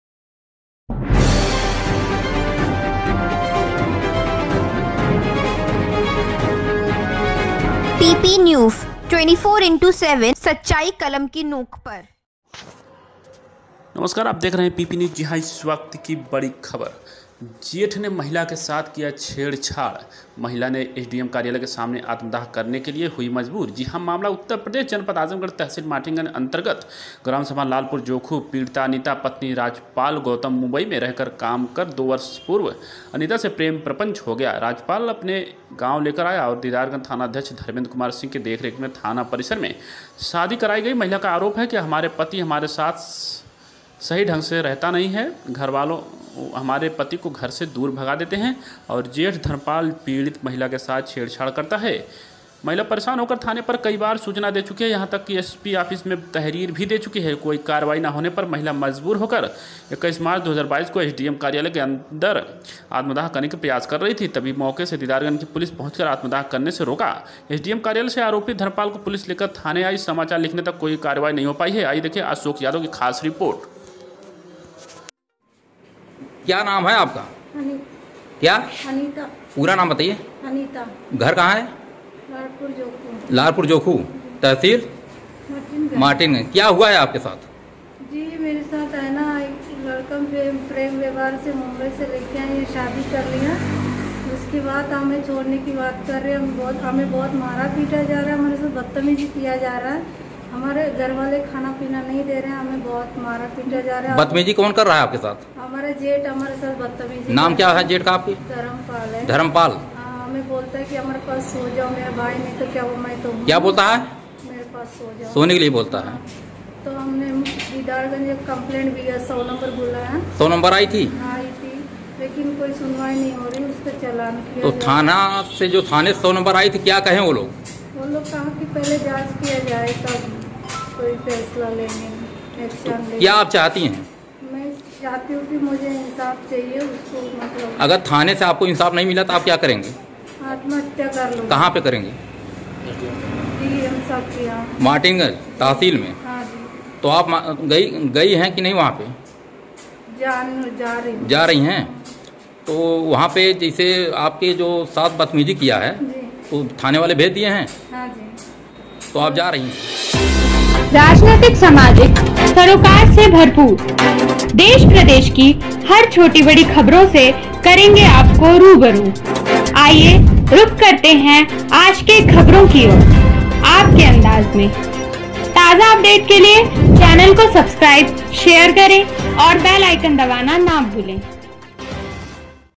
PP न्यूज 24X7 जेठ ने महिला के साथ किया छेड़छाड़ महिला ने एसडीएम कार्यालय के सामने आत्मदाह को मजबूर..
जेठ ने महिला के साथ किया छेड़छाड़ महिला ने एसडीएम कार्यालय के सामने आत्मदाह करने के लिए हुई मजबूर जी हां मामला उत्तर प्रदेश जनपद आजमगढ़ तहसील मार्टिनगंज अंतर्गत ग्राम सभा लालपुर जोखू पीड़िता अनीता पति राजपाल गौतम मुंबई में रहकर काम कर 2 वर्ष पूर्व अनीता से प्रेम प्रपंच हो गया राजपाल अपने गांव लेकर आया और दीदारगंज थाना अध्यक्ष धर्मेंद्र कुमार सिंह की देखरेख में थाना परिसर में शादी कराई गई महिला का आरोप है हमारे पति हमारे साथ सही ढंग से रहता नहीं है घरवालों हमारे पति को घर से दूर भगा देते हैं और जेठ धर्मपाल पीड़ित महिला के साथ छेड़छाड़ करता है महिला परेशान होकर थाने पर कई बार सूचना दे चुकी है यहां तक एसपी ऑफिस मैं तहरीर भी दे चुकी है कोई कार्रवाई ना होने पर महिला मजबूर होकर 21 मार्च 2022 को एसडीएम कार्यालय के अंदर आत्मदाह करने की प्रयास कर रही थी तभी मौके से दीदारगंज की पुलिस पहुंचकर आत्मदाह करने से रोका एसडीएम कार्यालय से आरोपी धर्मपाल को पुलिस लेकर थाने आई समाचार लिखने तक कोई कार्रवाई नहीं हो पाई